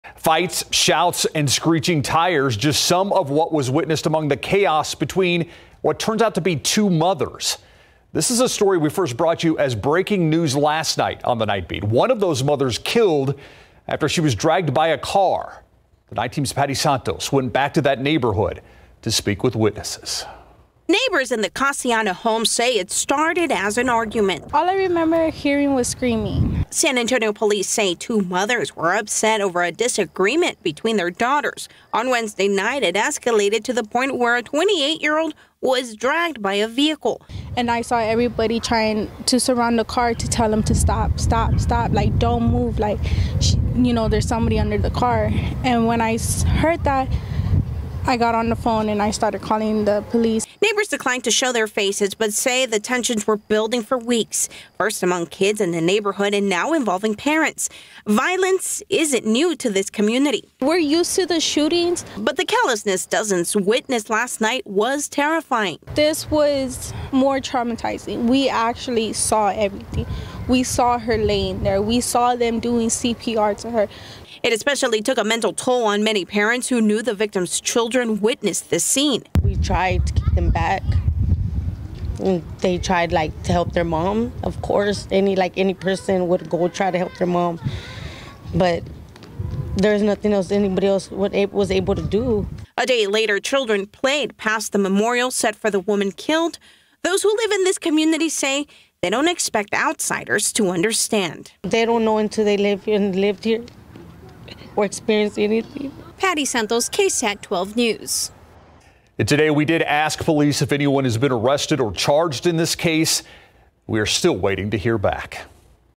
Cassiano Homes community traumatized by callous killing of woman
Dozens of people witnessed a Cassiano Homes resident being dragged by a vehicle before she was declared dead in the middle of the street minutes later.